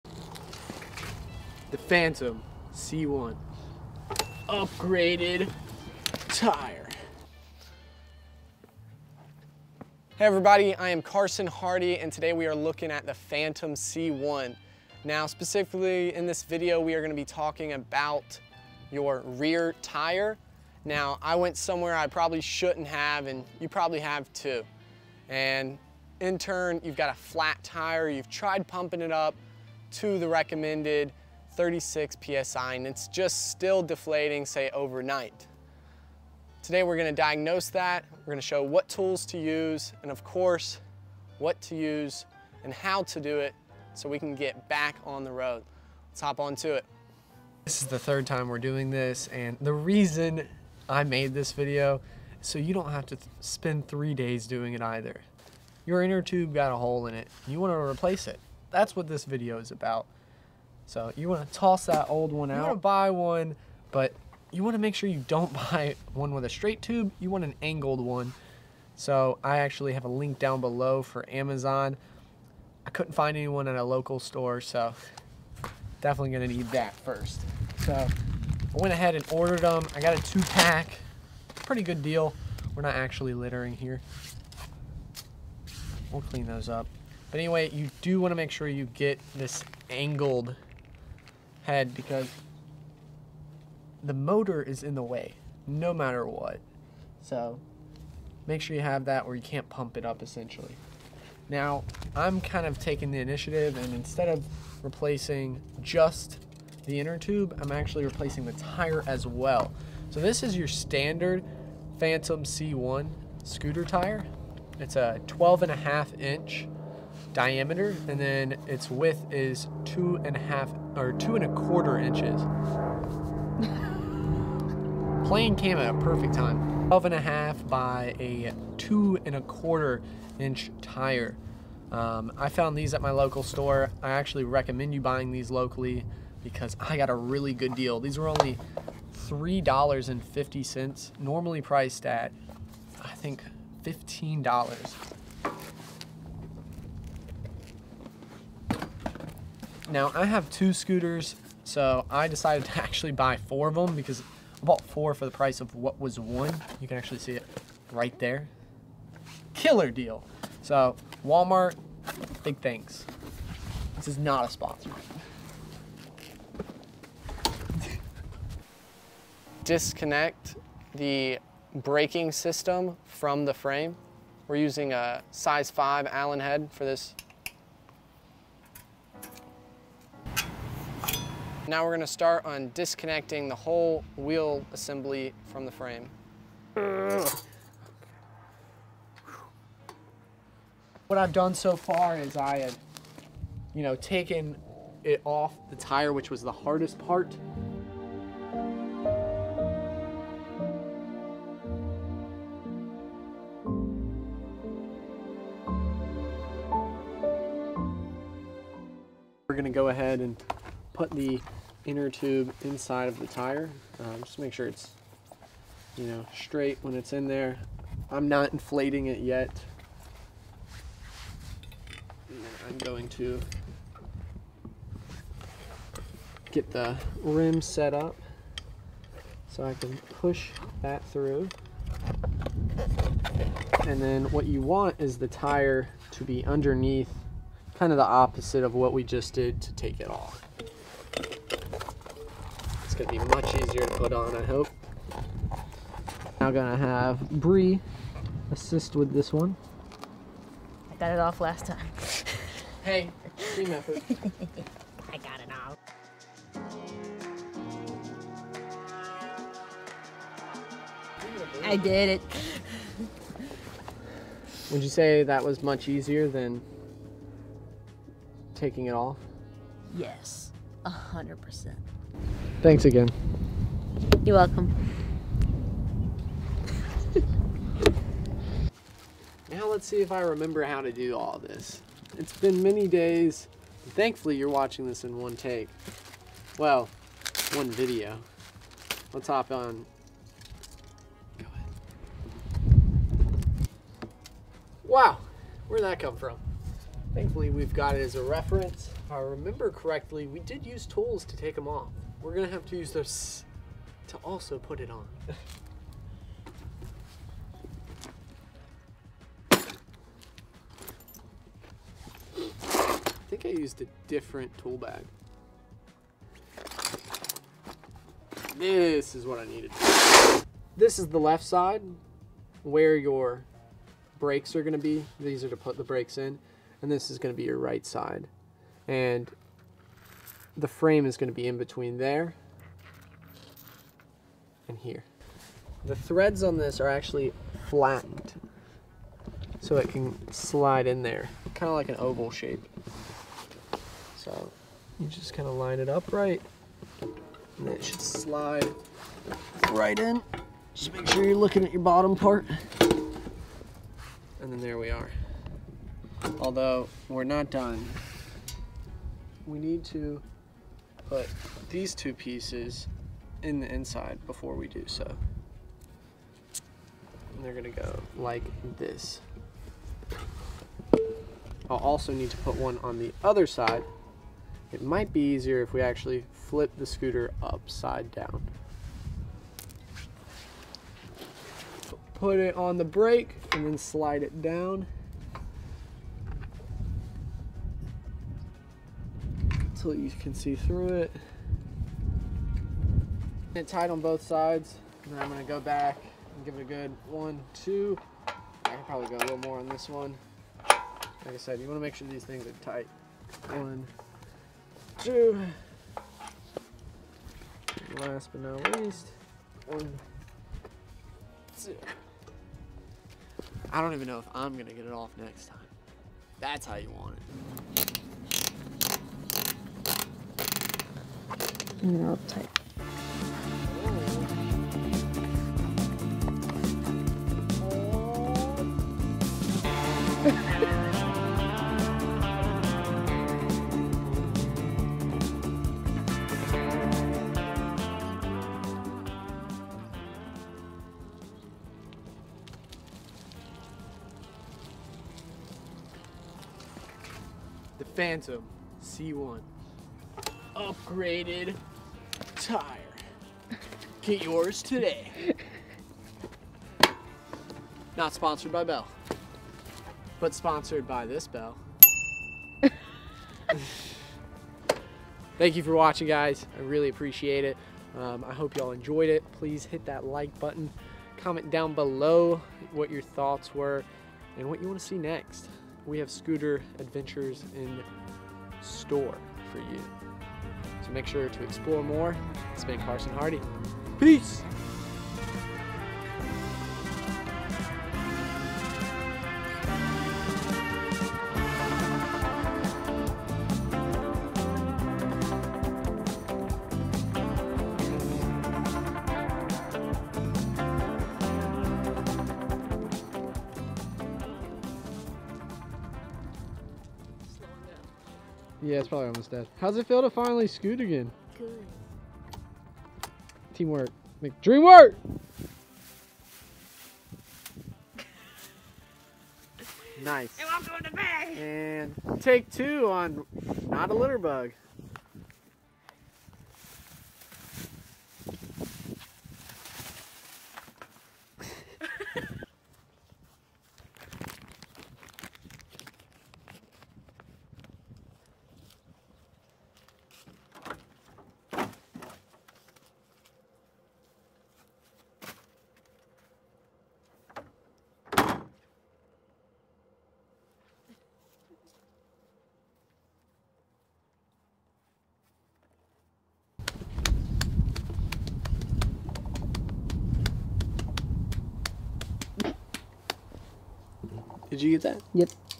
How to Fix an Electric Bike Tire? - Phantom C1 Upgrade
Angled Inner Tube (2 Pack)
Bell BMX Tire
I've definitely been riding too much... Just kidding that's never a thing, although it's time to fix/replace my inner tube and upgrade my tire while I'm at it. Off-road riding is much better with traction.

Scooter Bike

0:00 Intro
0:47 Words of Advice
2:49 Removing the Tire
4:51 Installing Tube & Tire
7:02 Test Ride!
7:45 Outro and Bloopers

GEAR USE:
Canon EOS R
Canon Advanced Lens Kit
Canon 24-70mm f4